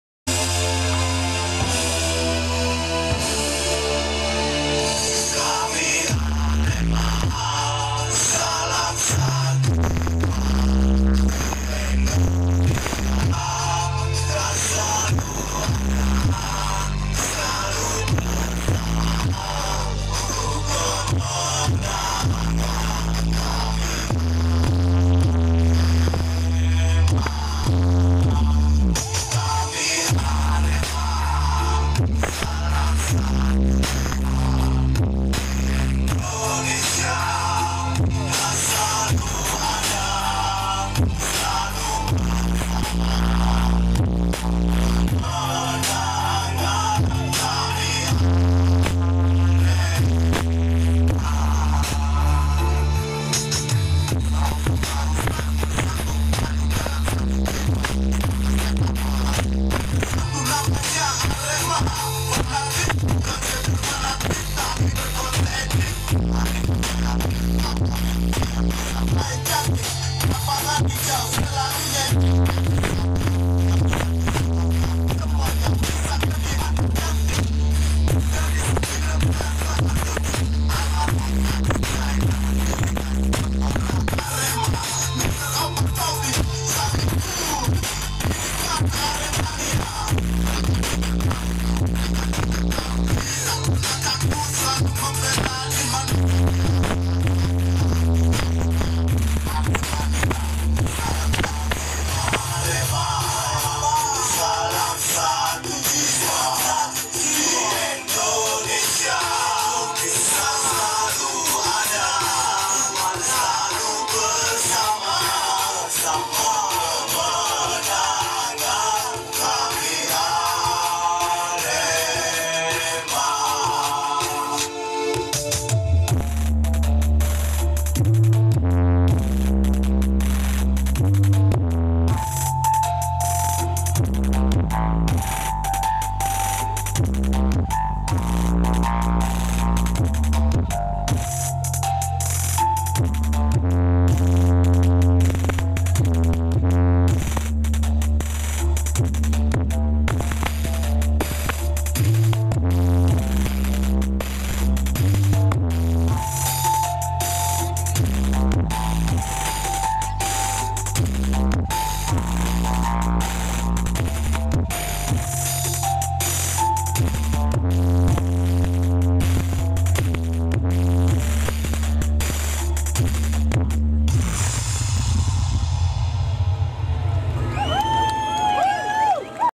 Bikin Merinding ...!!!! 4 Sound Putar Lagu Arema Bersamaan Di Wates Gondanglegi
~selamat datang di youtube saya🔵
Lagu arema#soundsystemmalang#watesgondanglegi

Terima kasih buat kalian
~Jangan Lupa~

 ✓Jagan lupa klik tombol lonceng biar tidak ketingalan vidio selanjutnya makasih🤗